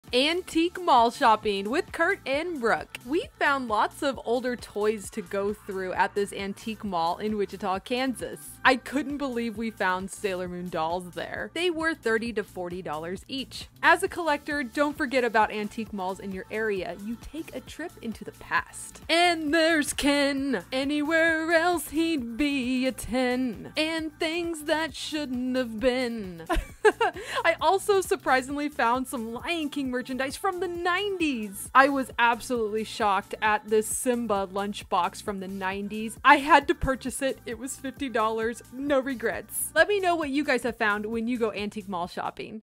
Antique Mall Shopping - Sailor Moon, Barbie, Disney & more!
We couldn't believe what we found! I guess we need to go antique shopping more often!

Stay up to date on the latest Sailor Moon merch & news

💝 Follow & Support 💝

SOCIALS
@ochibawolf everywhere